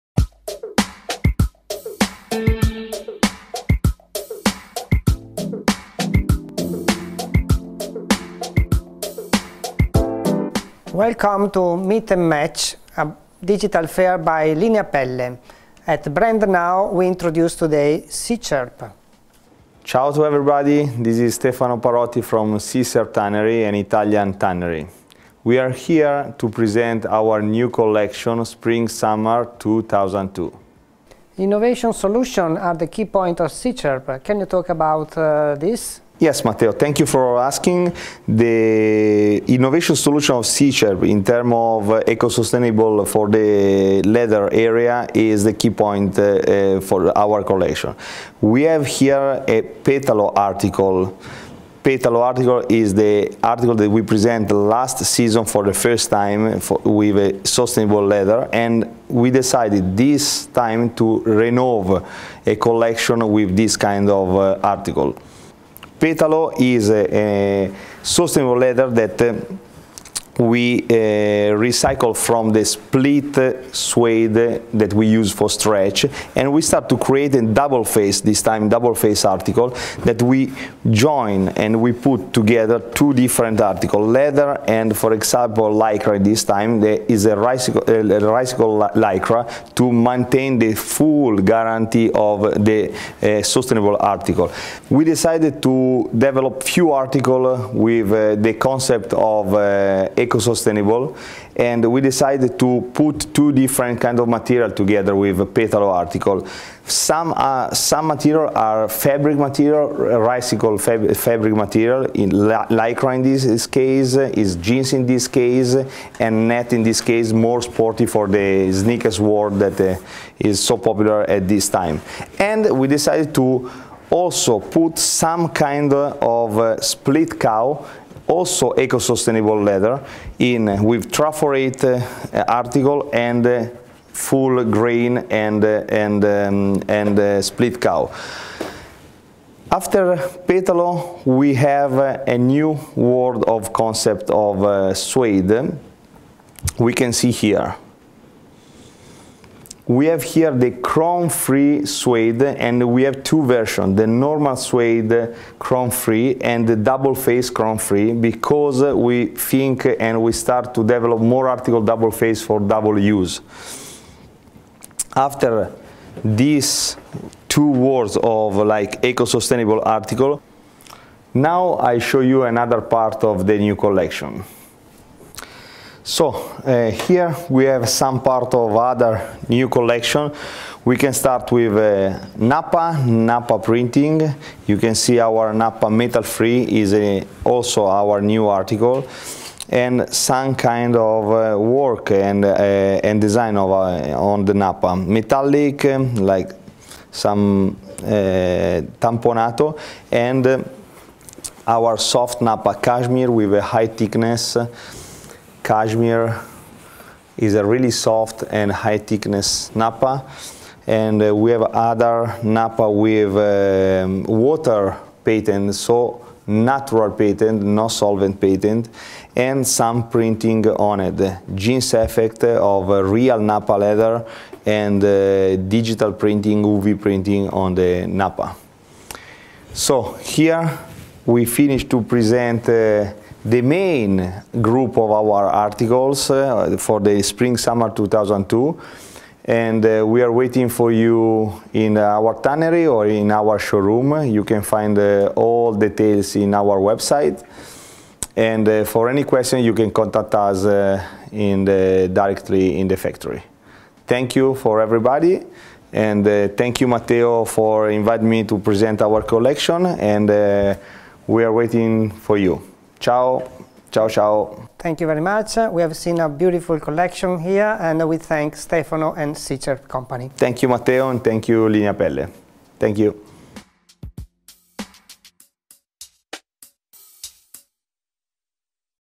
Brand Now - SICERP SPA
For almost 50 years, high quality leathers for leathergoods and footwear, destined for the fashion and luxury sector. It offers a product entirelt "made in Italy" obtained with artisan experience but, at the same time, attentive to new technologies and respect for the environment.